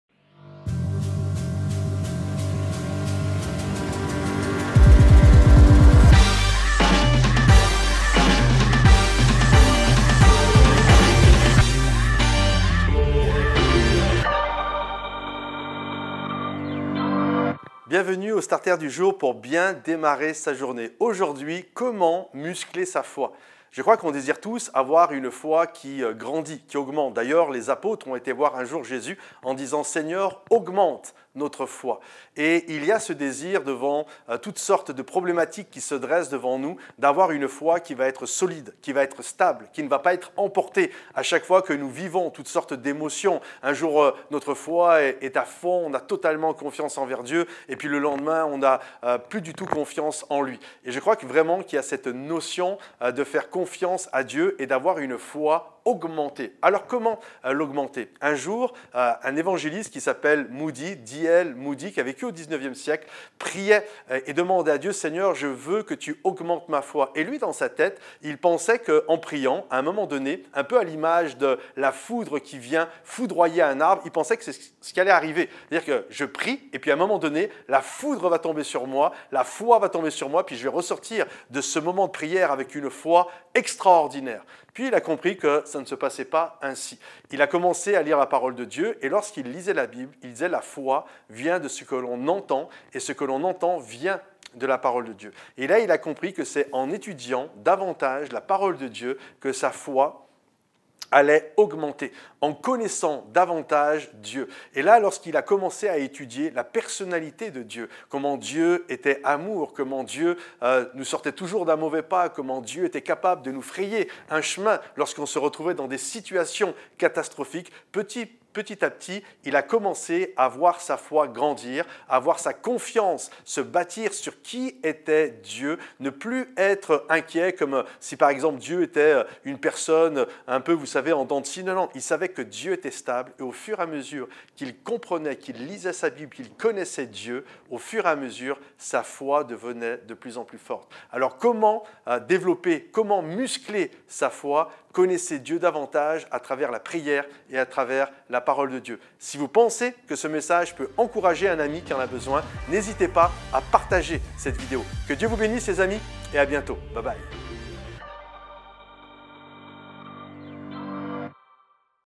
STARTER - Comment muscler sa foi ?
La foi n'est pas une chose qui tombe sur nous de manière extraordinaire, une fois pour toutes... non, notre foi augmente lorsque nous apprenons, petit à petit, à connaître notre Dieu intimement dans la prière et dans la lecture de sa Parole. Ainsi, nous réaliserons toujours davantage que Dieu est stable, au contrôle, et qu'il ne nous abandonnera pas.
FAIRE UN DON À L'ÉGLISE :
RESTONS CONNECTÉS !